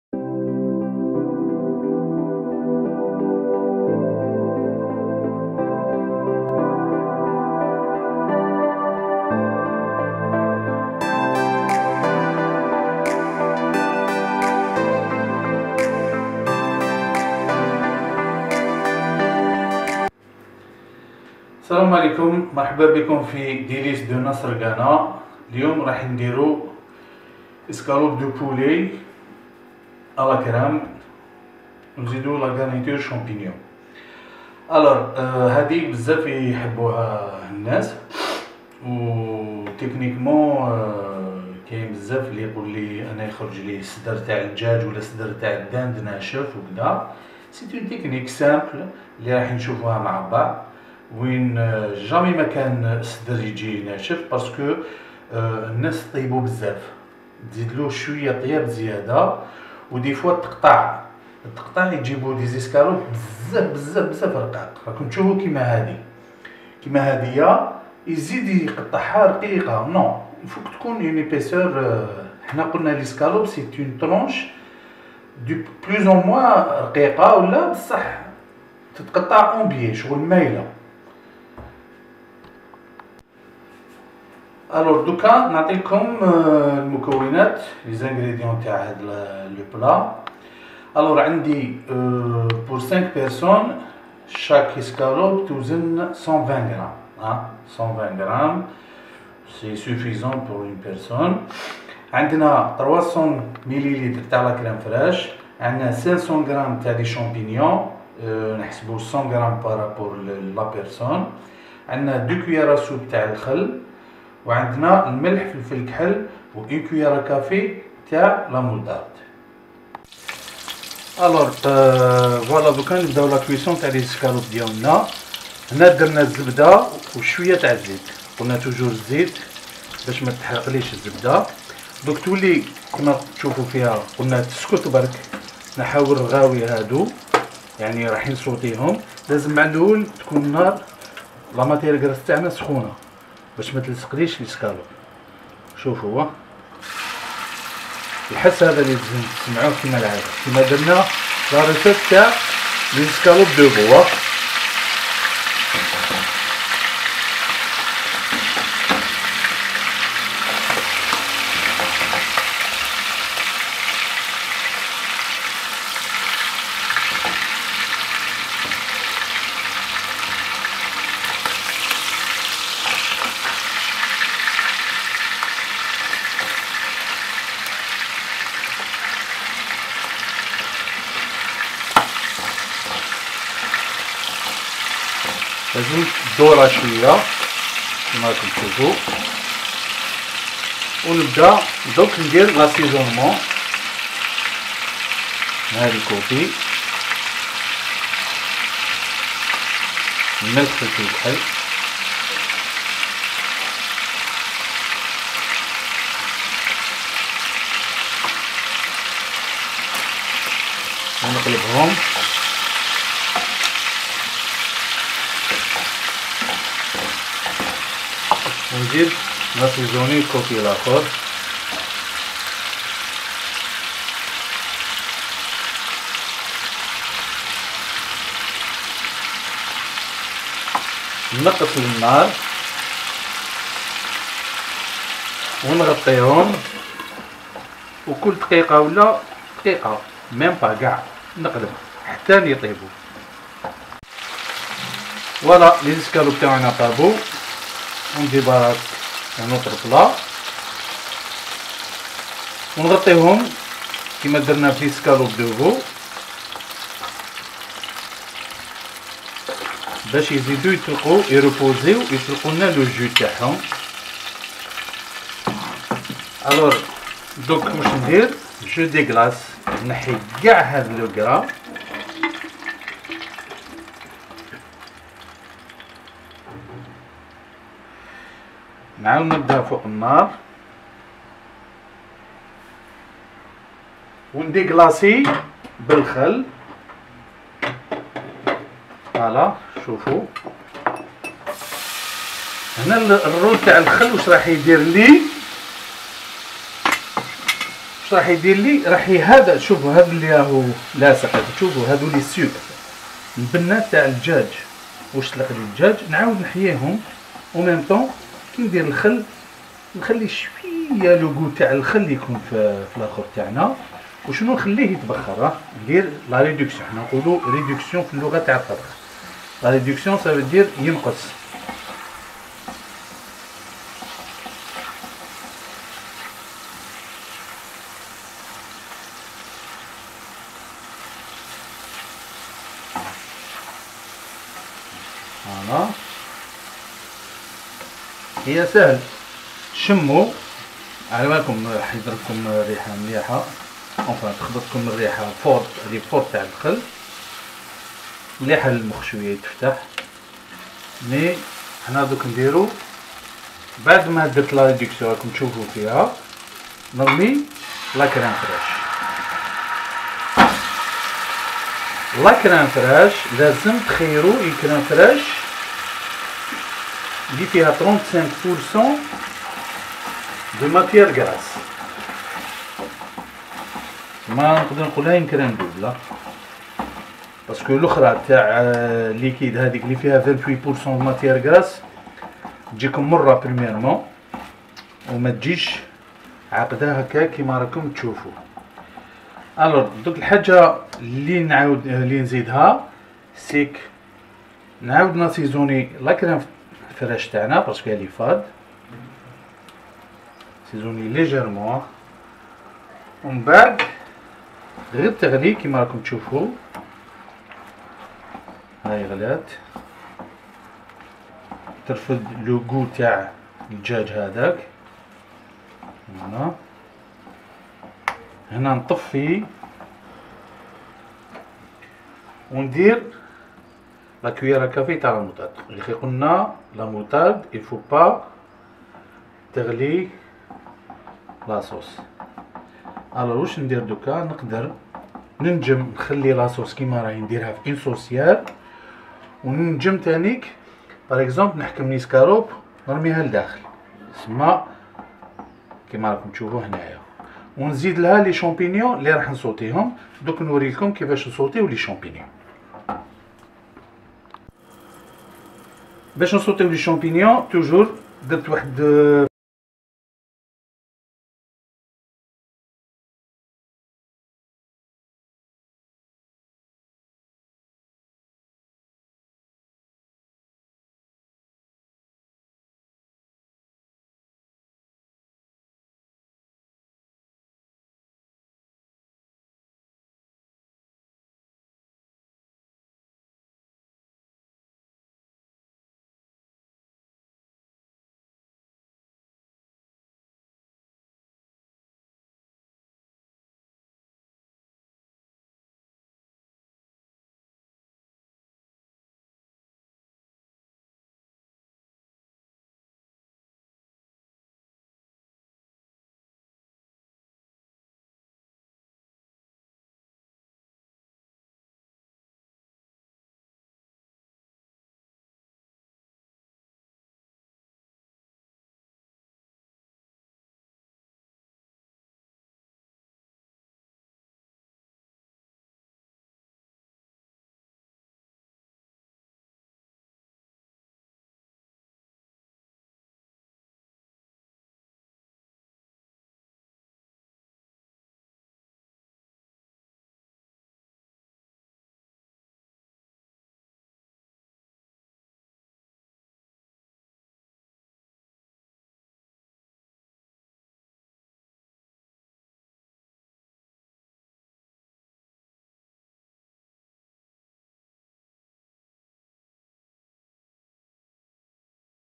escalopes de poulet à la crème très facileشرائح الدجاج بالقشدة سهلة جدّا
ingrédients: 500g d'escalopes de poulet, 500g de champignons, 1 c à café de moutarde, 300 ml de crème fraîche à 35% de matière grasse, 2 c à soupe de vinaigre, sel et poivre, beurre et huile pour la cuisson